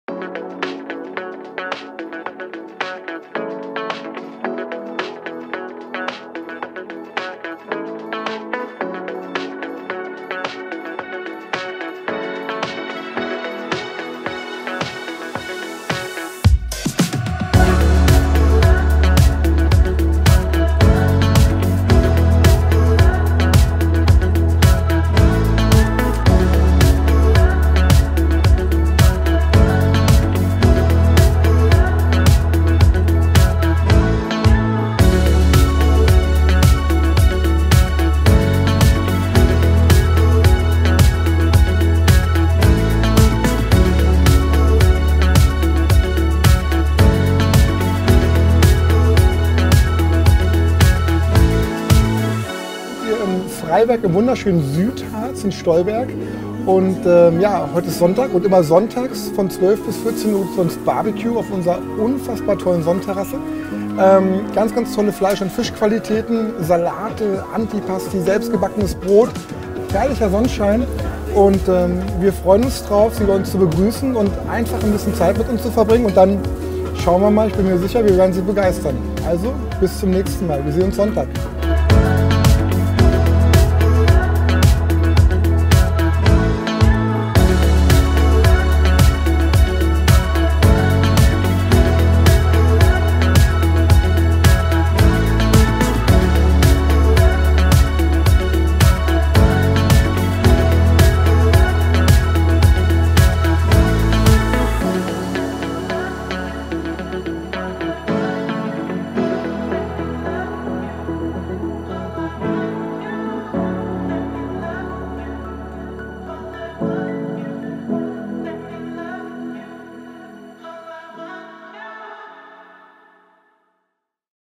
SUNDAY BBQ auf der schönsten Sonnenterasse im Südharz | Romantik Hotel FreiWerk
Sie waren noch nicht bei unserem SUNDAY BBQ? 🍗🥩🍔⁠
Na dann wird es aber Zeit! ⏰⁠
Noch bis September verwöhnen wir Sie jeden Sonntag mit unserem exklusiven BBQ auf der Sonnenterasse. ⁠

Freuen Sie sich auf eine Vielzahl an Vorspeisen, das BBQ mit ganz verschiedenen Köstlichkeiten und kleinen, süßen Desserts. ⁠
Wir wünschen Ihnen schon jetzt guten Appetit und freuen uns auf Ihren Besuch! 😋⁠